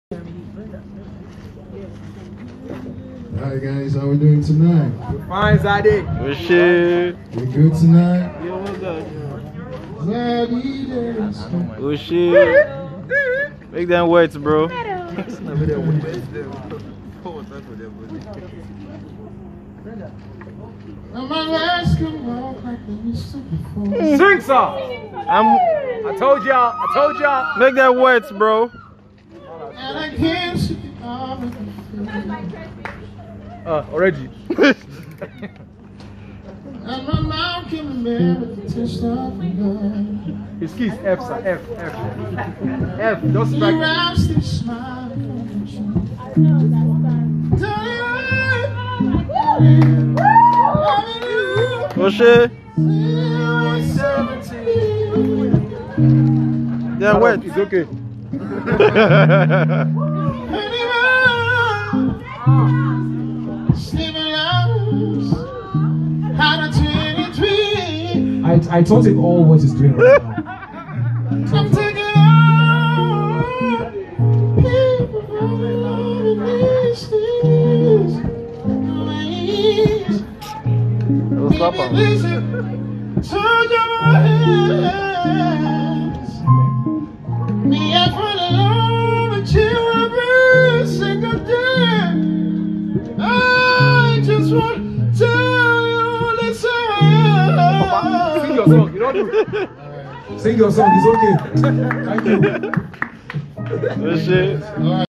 Zadok- Expressing himself with some RnB soul vibes
ITS BEEN A WHILE FROM THE SOUL AND PITCH KING ZADOK...
GUYS ENJOY SOMETHING LIGHT.